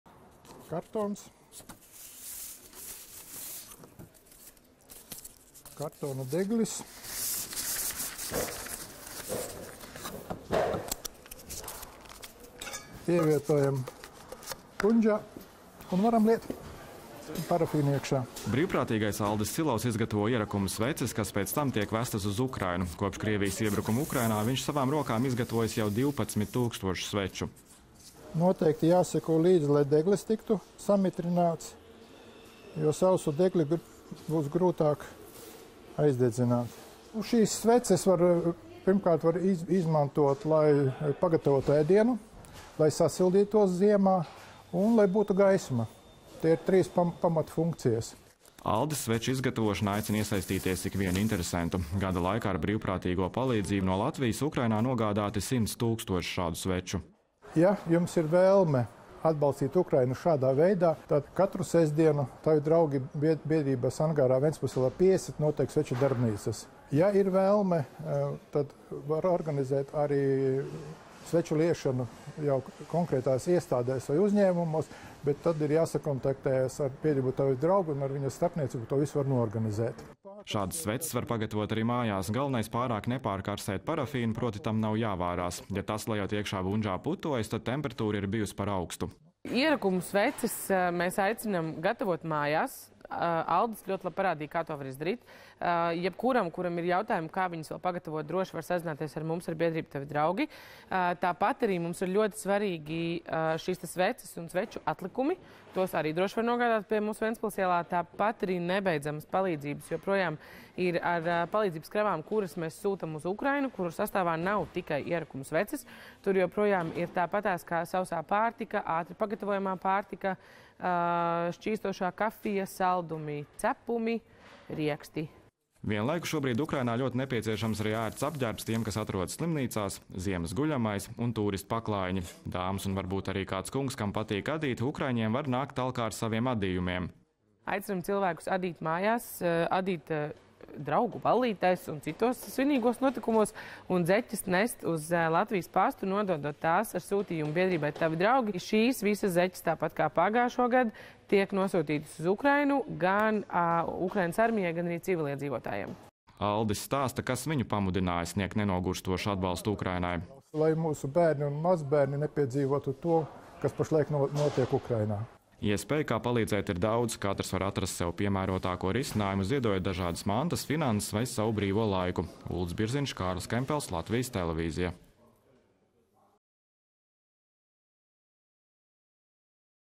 Aicina palīdzēt sagatavot ierakumu sveces Ukrainai
Iestājoties ziemas sezonai, ikviens iedzīvotājs tiek laipni aicināts palīdzēt sagatavot ierakumu sveces, ko sūtīt uz Ukrainu. Tas ukraiņu karavīriem palīdz gan sasildīties, gan pagatavot ēdienu. Kā šādas sveces iespējams pagatavot un kā vēl varam palīdzēt ukraiņiem?